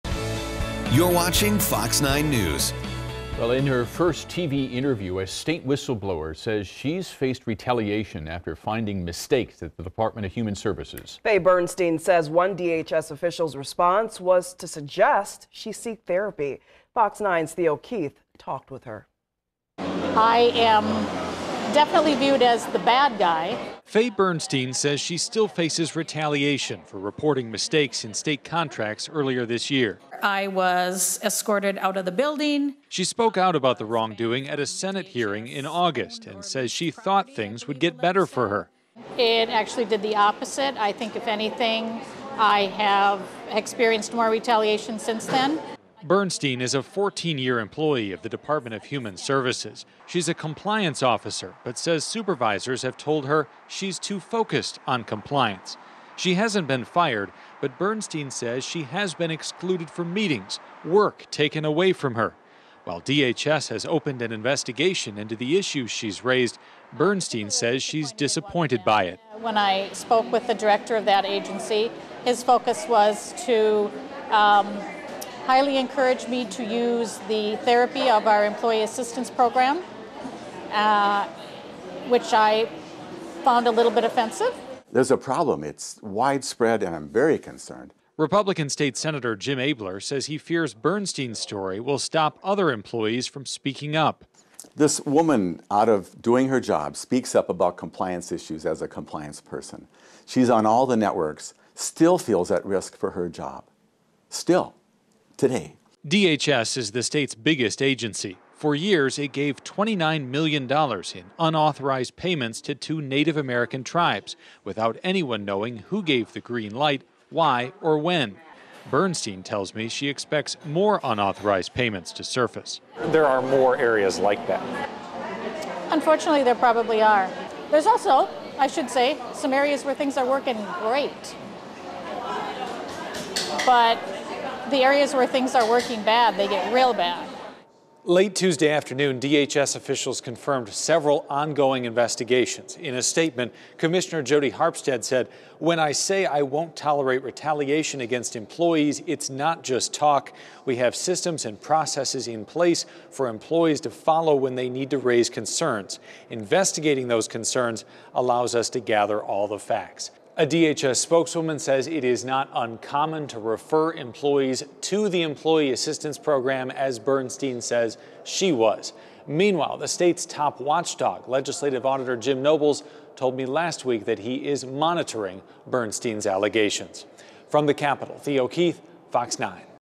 Minnesota DHS Whistleblower says She Faces Retaliation, Told to Seek Therapy
I did the first TV interview with a whistleblower at the embattled Minnesota Department of Human Services.